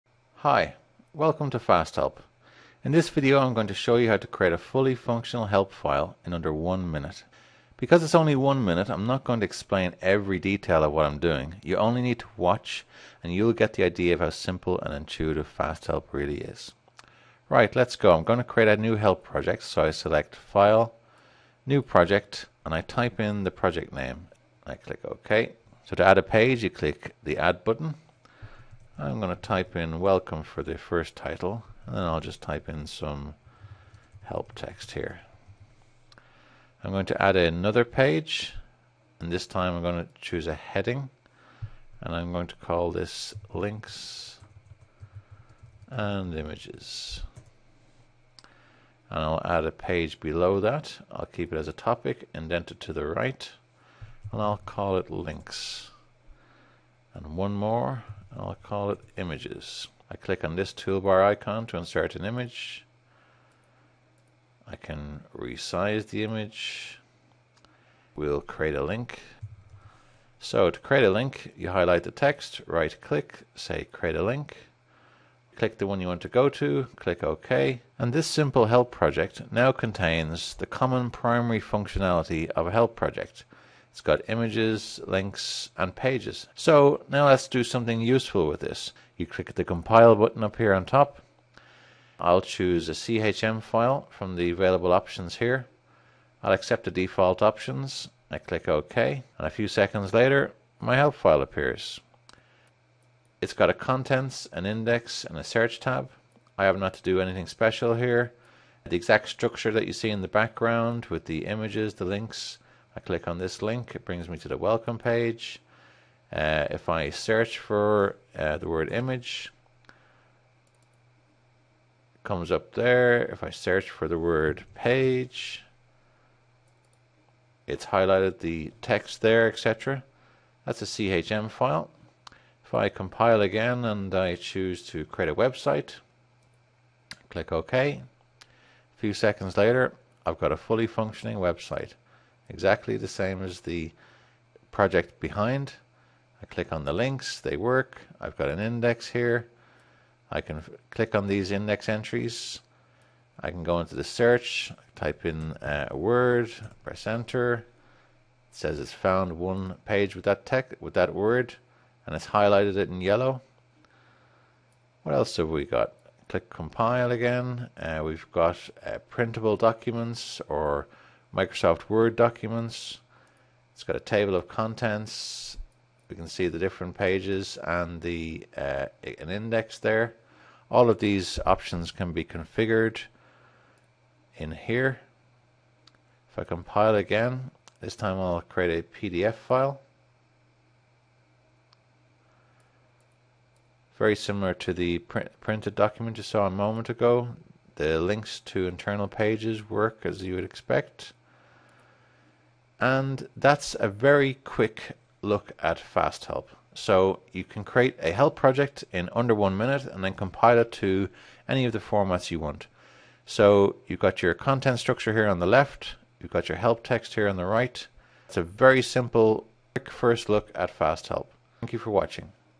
Overview of FastHelp
Shows how to create a basic help project in one minute, then compile to multiple formats, such as CHM file, Website Help and PDF.